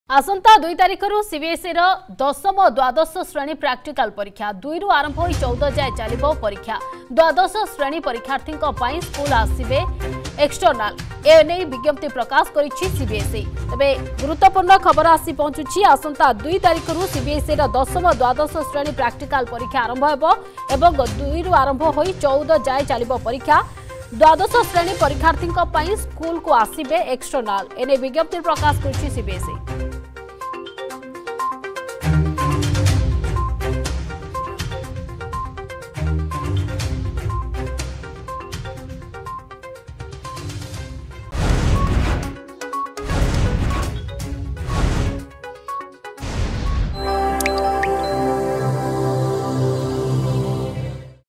CBSE Board Exam 2023: Practical Examinations From January 2
CBSE Board exam 2023: Practical examination from January 2.